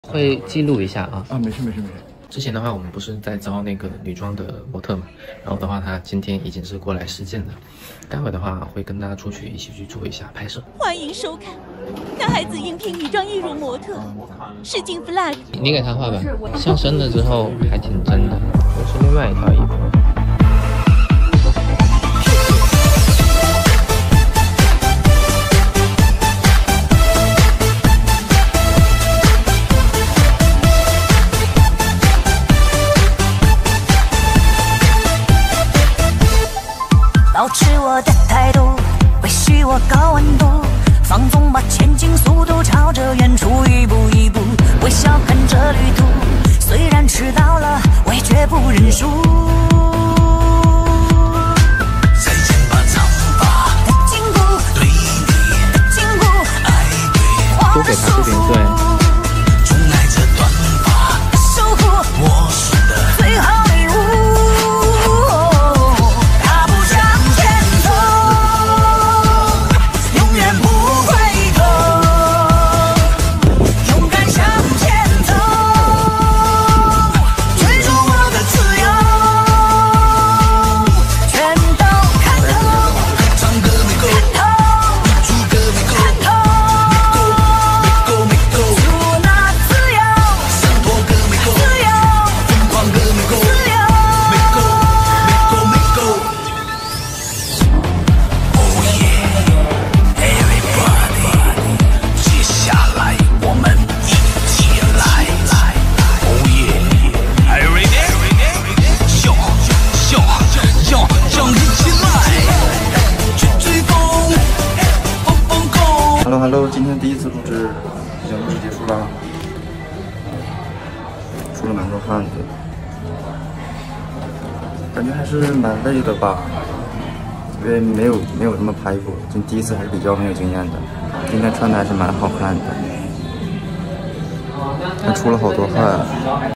A boy applies for the first VLOG job as a women's prosthetic breast model#drag
男孩子第一次应聘女装义乳模特VLOG